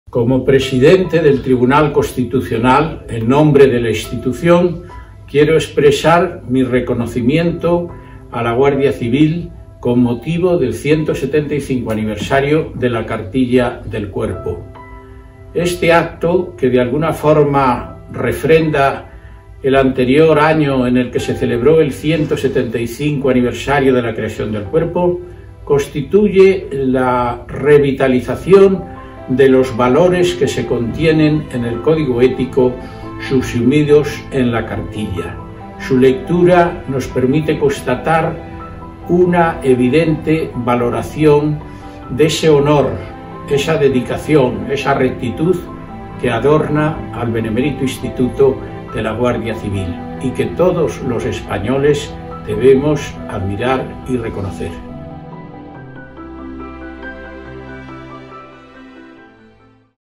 Juan José González Rivas, Presidente Tribunal Constitucional, 175 años de la Cartilla
Palabras de Juan José González Rivas,  Presidente Tribunal Constitucional, 175 años de la Cartilla del Guardia Civil